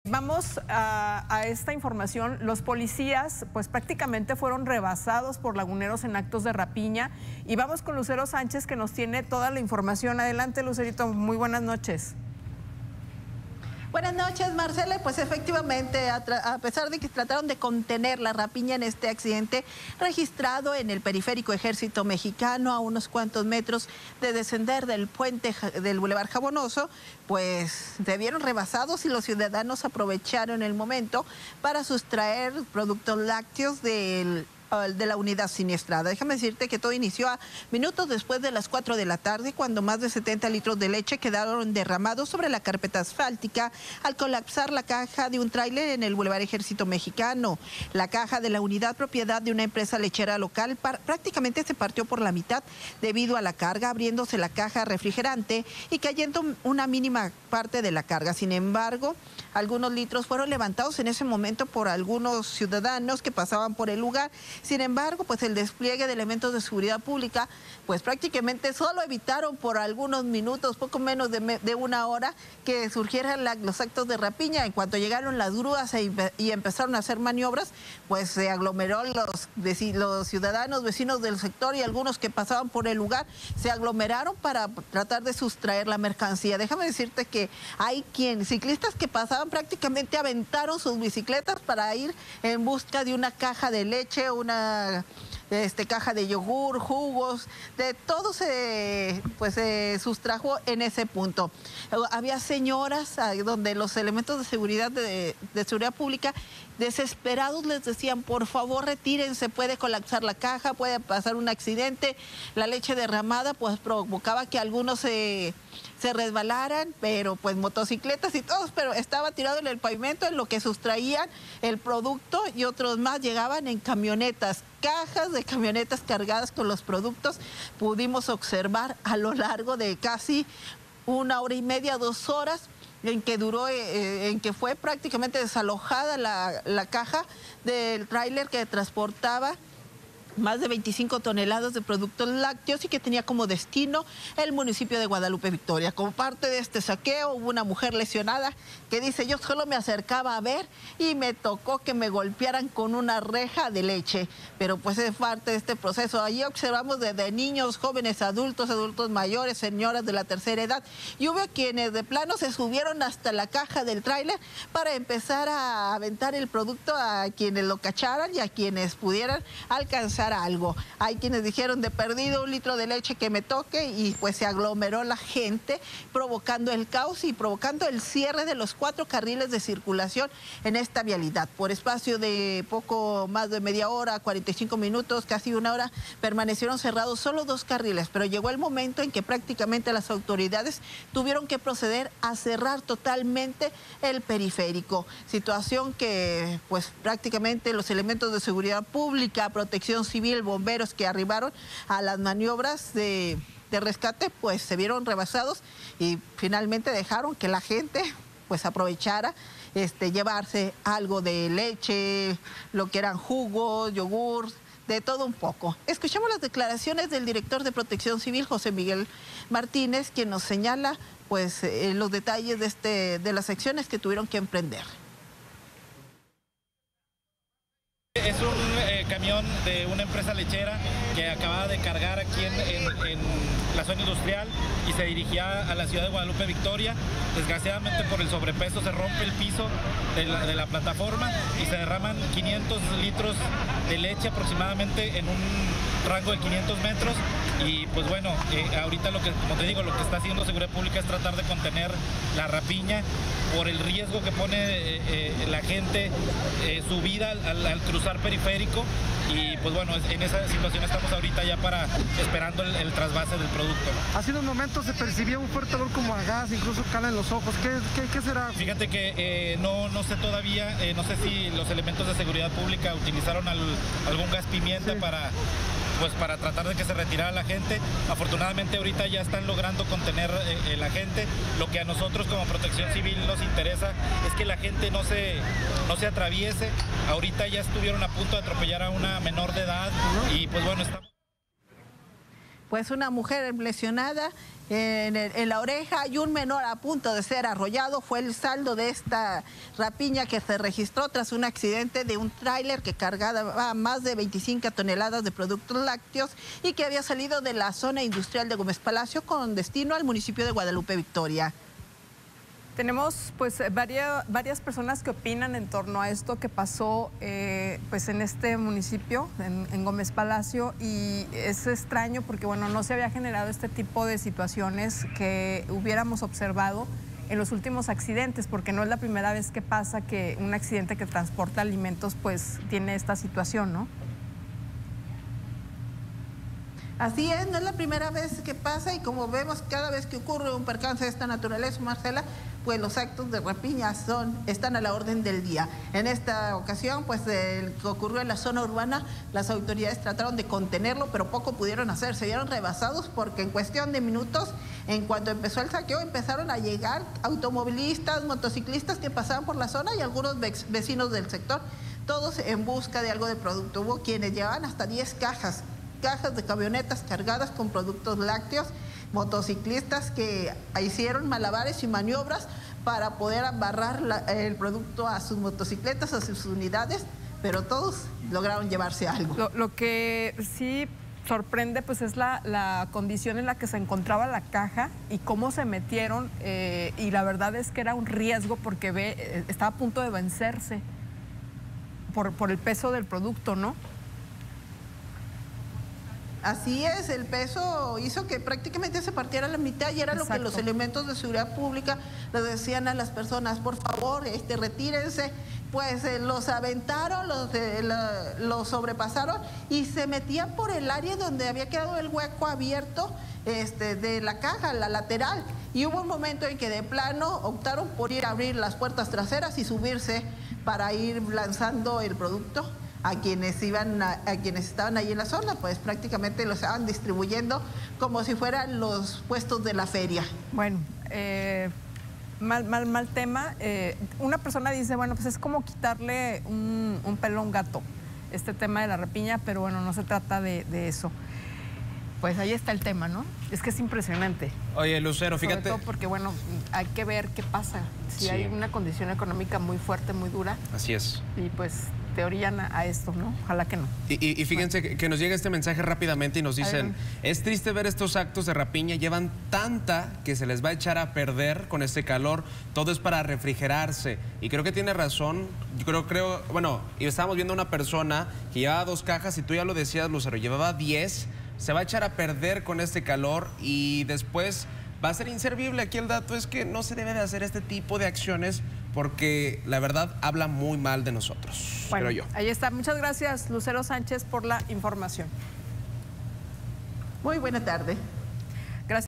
Impactante rapiña tras colapso de tráiler de leche
Un tráiler con productos lácteos colapsó sobre el bulevar Ejército Mexicano en Gómez Palacio. La estructura, a pesar de no ser segura, no detuvo a los laguneros para realizar una rapiña.